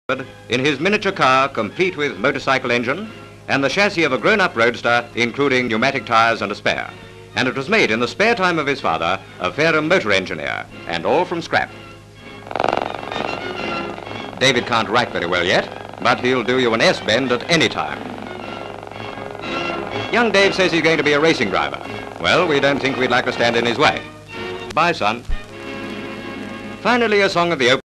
Kids Car (1941)
Kids car.

Various shots of 5 year old David Hansford in a little car with a motorcycle engine.  He weaves between some flags.  M/S of him at the wheel, he waves.
 FILM ID:1538.04